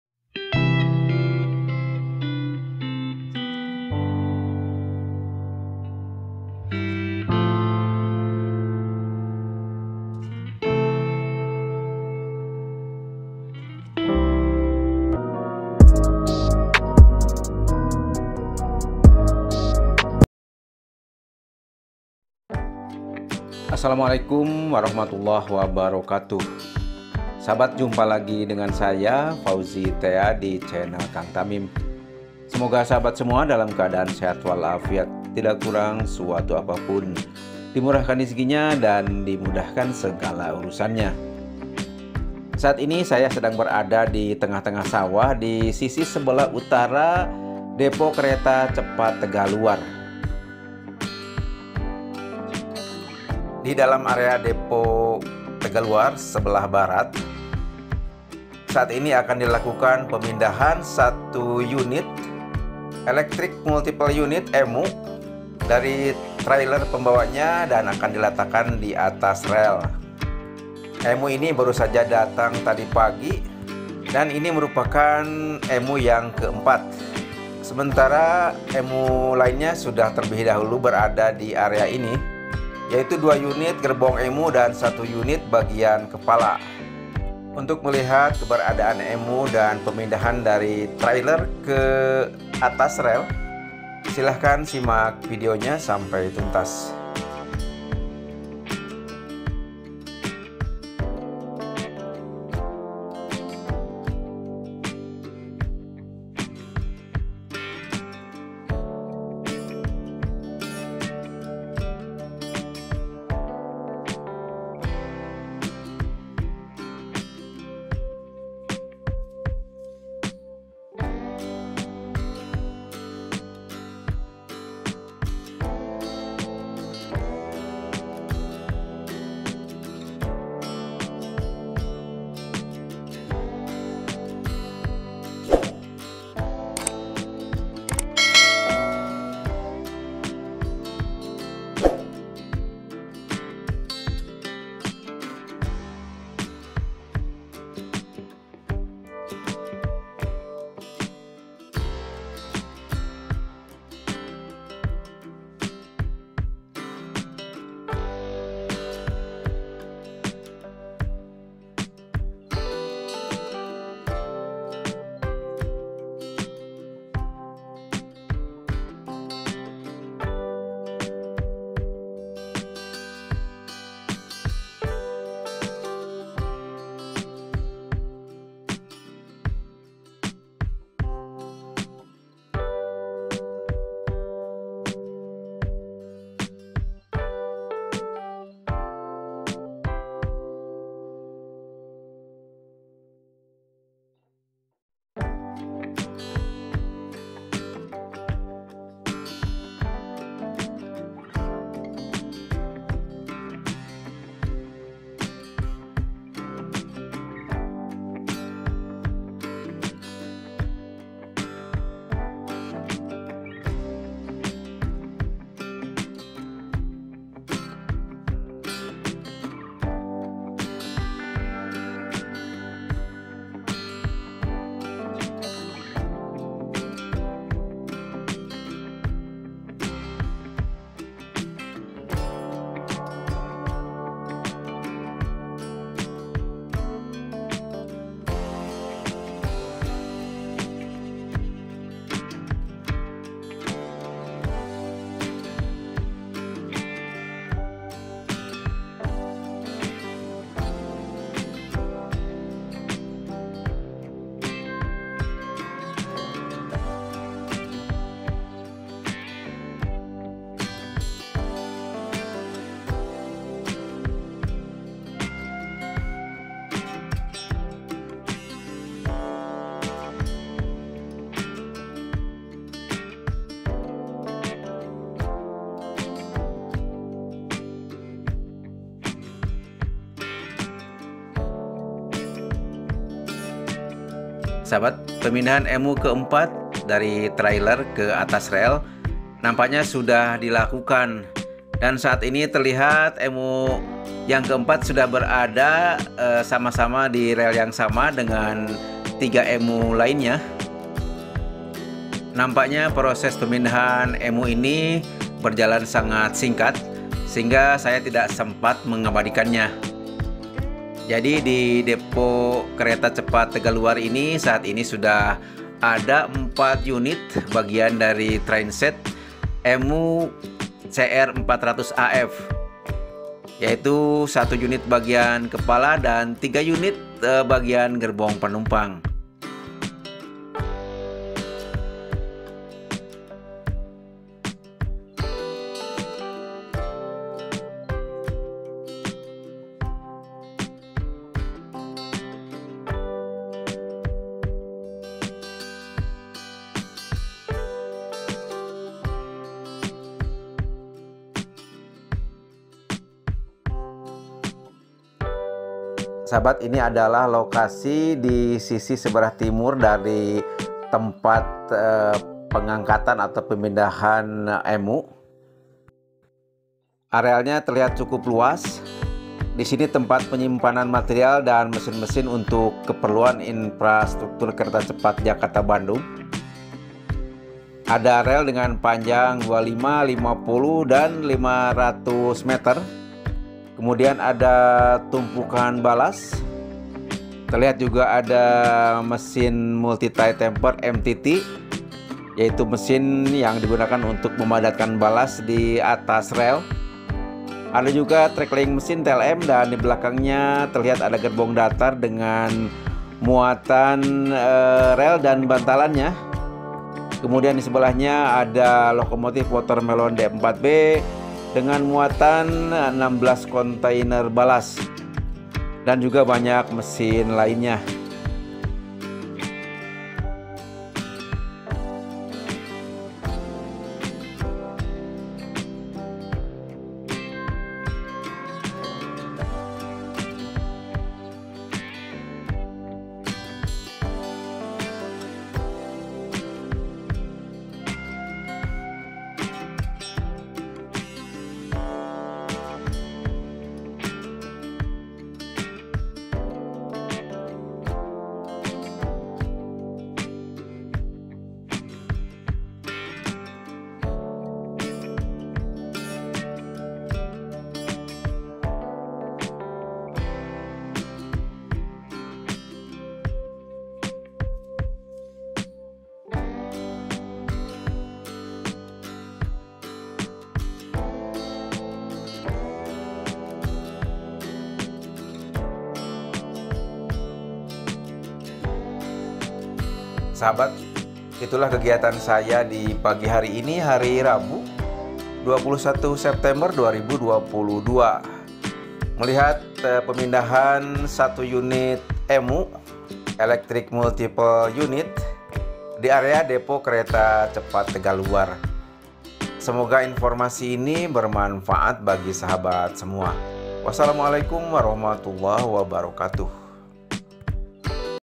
EMU Ke 4 Sudah Dipndah Ke Rel - Total 4 Unit EMU Ada Di Depo Tegalluar
Bagian dari trainset EMU Kereta cepat jakarta bandung ke 4 baru saja tiba pagi tadi jam 6.00, hari rabu 21 September 2022 dan saat ini di Depo tegalluar sudah berada 4 unit EMU, satu unit bagian kepala dan 3 unit bagian gerbong penumpang